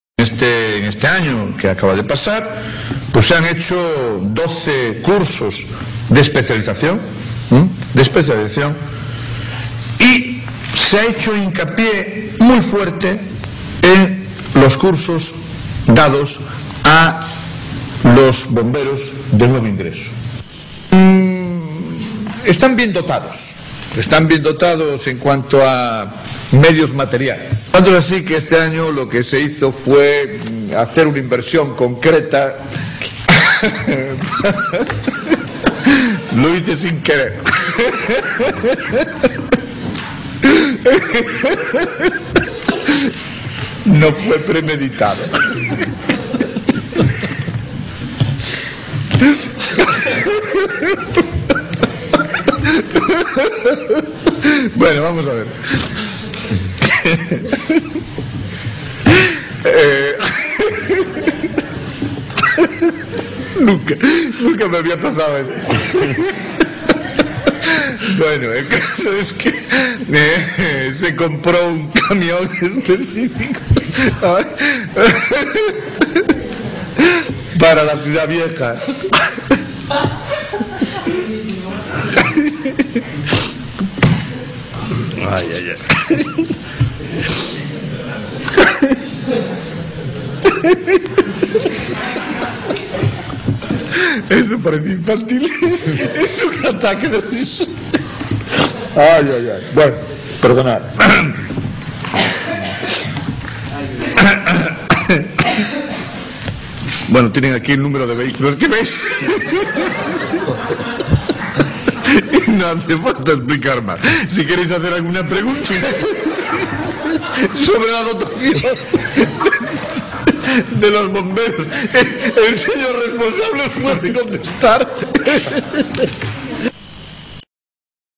Un ataque de Risa en una rueda de prensa
Le da un ataque de risa mientras habla de las nuevas dotaciones de los bomberos :D